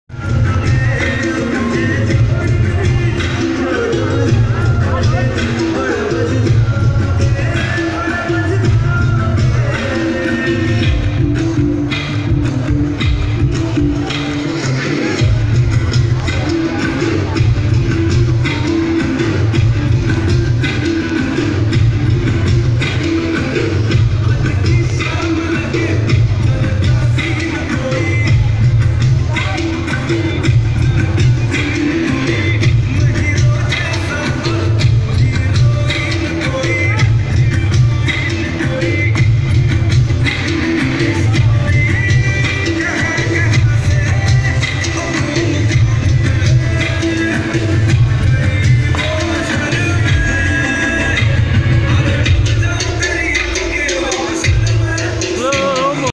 Mei Kaushik Patel. Mera "Dodhiya" style ka garba ka video.
Video from patelkaushik117

Mei Kaushik Patel ,
Surat...
Date:: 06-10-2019, Sunday, Navraatri ki, Mata..ji ki Aatham ke din , " Dodhiya " Garba style ,10 step wala , khela thaa oosiki video.
How like you my style??
Tell me...
OK?
By.